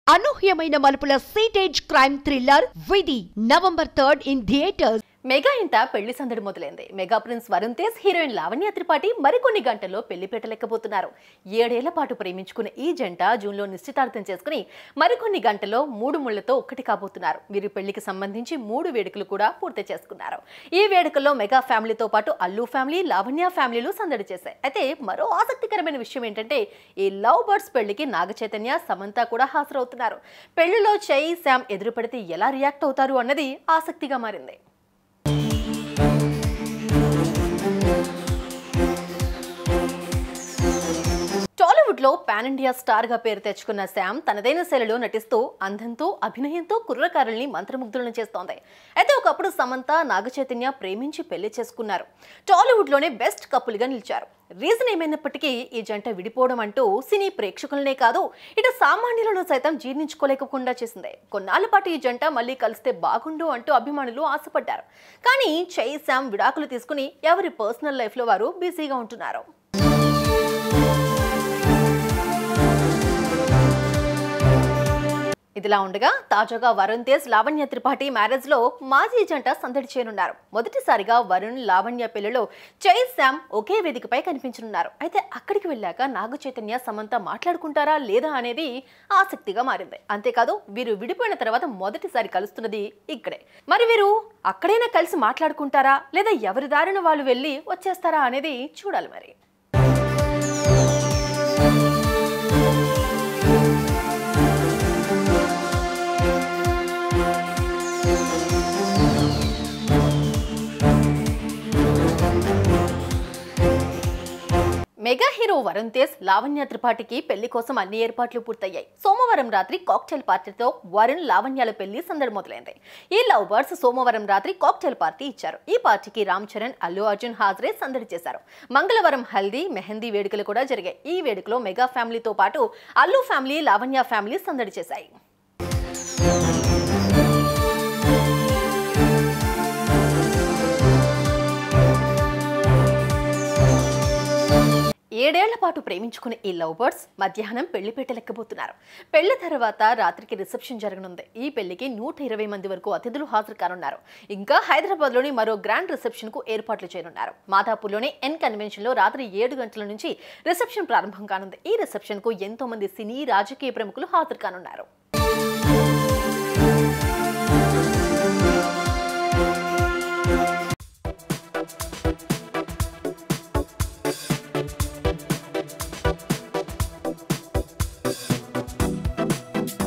విడాకుల తర్వాత మొదటిసారి కలవబోతున్న సమంతా నాగ చైతన్య | Varun Tej & Lavanya Tripathi Wedding
విడాకుల తర్వాత మొదటిసారి కలవబోతున్న సమంతా నాగ చైతన్య | Varun Tej & Lavanya Tripathi Wedding | Samantha | Naga Chaitanya | Sumantv Talkies